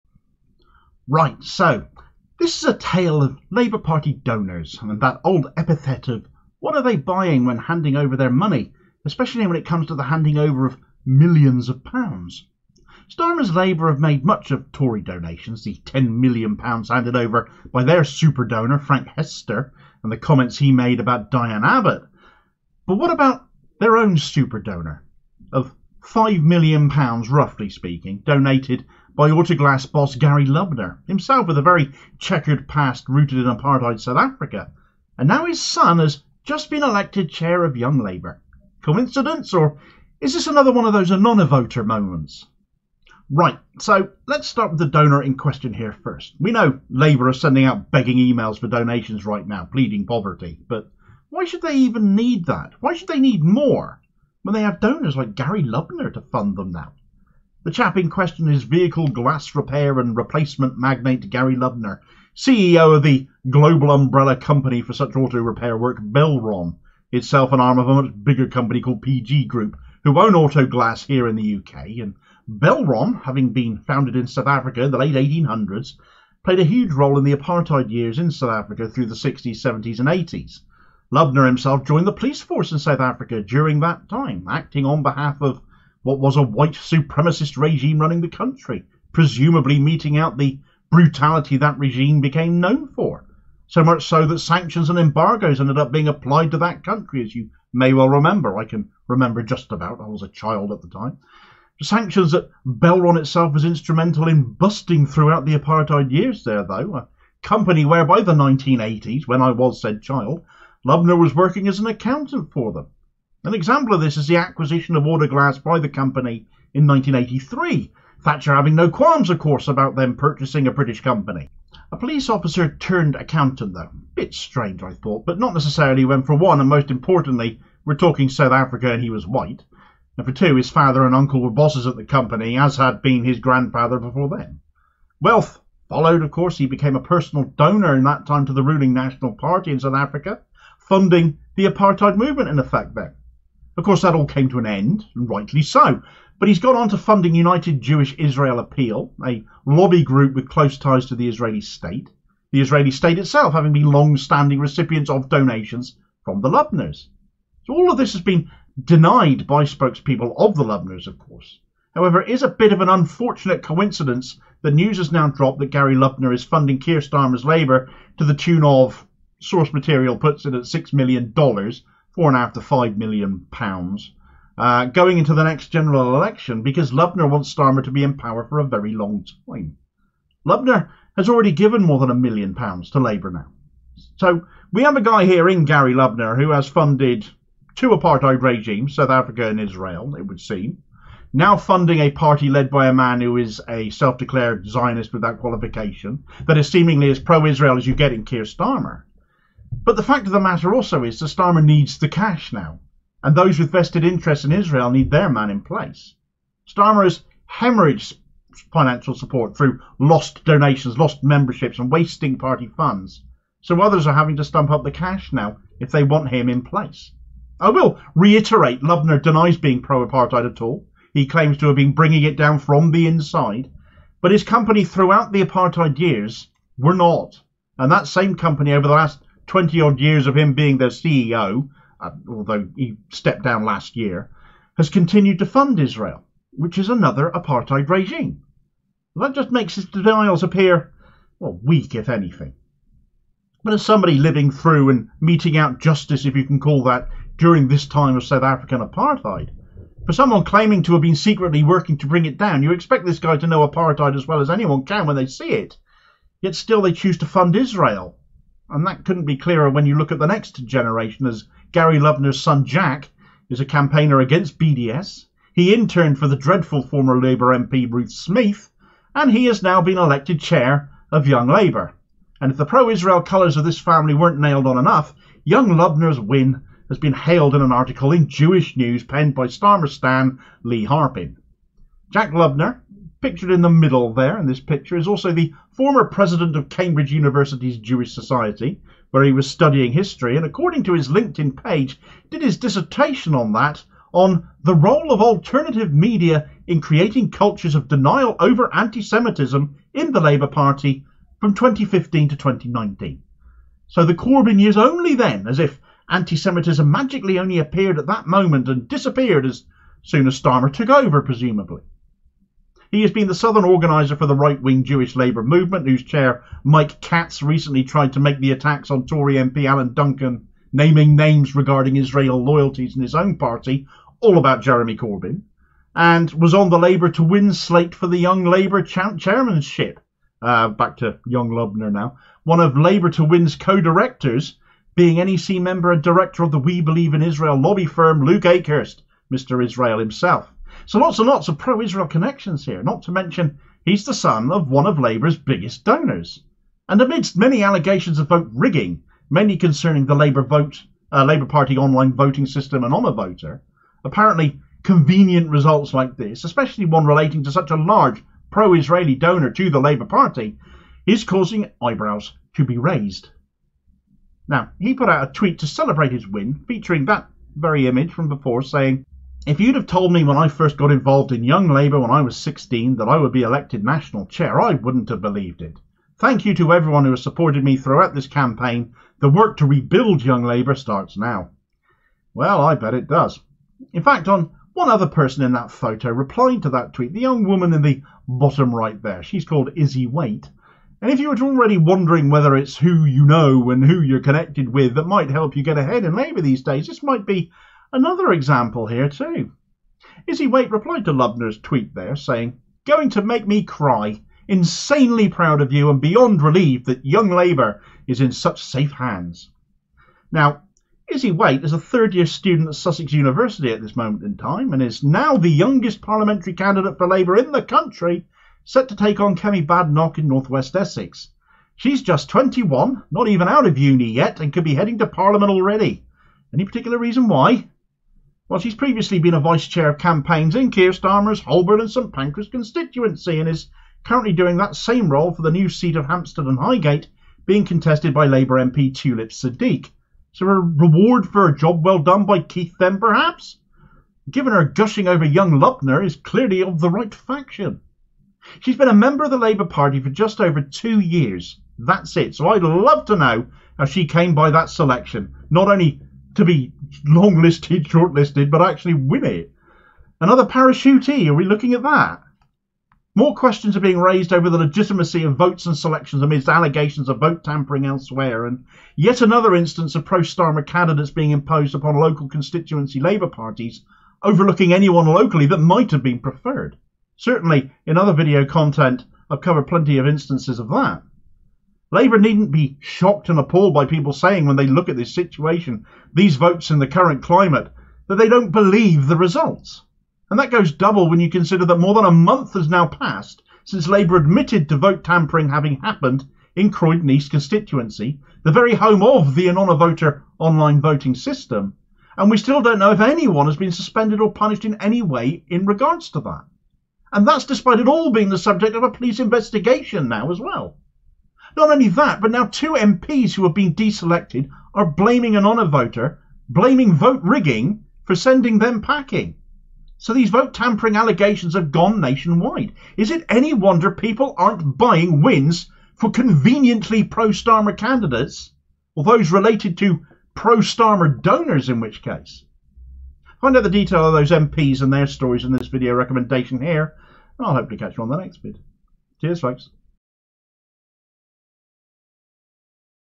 South African apartheid donor's son now leads Young Labour.
The son of a South African millionaire who has given millions to Starmer's Labour is the new chair of Young Labour.
Right, so this is a tale of Labour Party donors and that old epithet of what are the buying when handing over their money, especially when it comes to the handing over of millions of pounds. Starmer’s Labour have made much of Tory donations, the £10m handed over by their super donor Frank Hester, but what of their own super donor? £5m donated by Autoglass boss Gary Lubner, himself with a very checkered past rooted in apartheid South Africa and now his son has just been elected chair of Young Labour. Coincidence, or is this another one of those Anonyvoter moments?
Right, so let’s start with the donor in question here first, we know Labour are sending out begging emails for donations right now, but why should they need more when they have donors like Gary Lubner to fund them now?
The chap in question is vehicle glass repair and replacement magnate Gary Lubner, CEO of the global umbrella company for such auto repair work Belron, itself an arm of a bigger company, PG Group, who own Autoglass here in the UK and Belron, having been founded in South Africa in the late 1800s, played a huge roll in the apartheid years through the 1960s, 1970s and 1980s in that country. Lubner himself joined the police force in South Africa during that time, acting on behalf of the white supremacist regime running the country, presumably meting out the brutality that regime became known for, so much so that sanctions and embargoes were applied to that country, sanctions that Belron itself was instrumental in busting throughout the apartheid years there, a company where by the 1980s Lubner was working as an accountant for. An example of this is the acquisition of Autoglass by the company in 1983, Thatcher having no qualms about them purchasing a British company of course. A police officer turned accountant though? A bit strange, but not necessarily when for one and most importantly he was white and for two his father and uncle were bosses at the company, his grandfather had been as well before them. Wealth followed, he became a personal donor to that ruling National Party, funding the apartheid movement. Of course that all came to an end and rightly so, but he’s gone on to funding United Jewish Israel Appeal, a lobby group with close ties to the Israeli state, the Israeli state itself having been longstanding recipients of donations from the Lubners. All of this has been denied by spokespeople of the Lubners of course, however it’s a bit of an unfortunate coincidence that news has now dropped that Gary Lubner is funding Keir Starmer’s Labour to the tune of $6m going into the next election because Lubner wants Starmer to be in power for a very long time. Lubner has already given more than a million to Labour now. So we have a guy who has funded two apartheid regimes, now funding a party led by a man who is a self declared Zionist without qualification, that is as seemingly pro-Israel as you get?